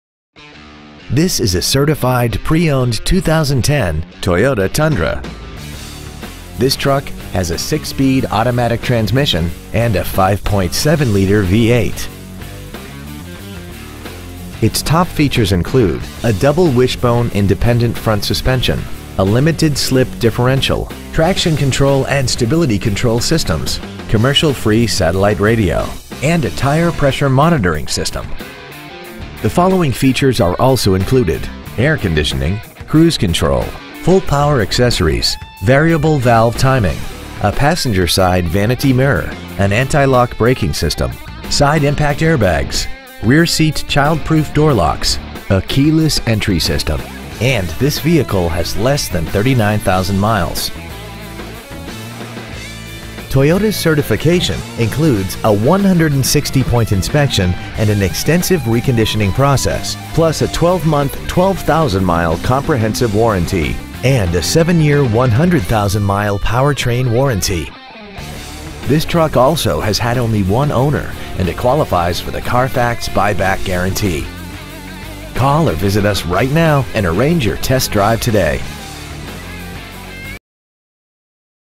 2010 Toyota Tundra 2WD Houston Texas 77034
Year : 2010
 Make : Toyota
 Model : Tundra 2WD
 Engine : Gas V8 5.7L/346
 Trans . : Automatic
 Exterior : Red
 Miles : 38,579
 Interior : Black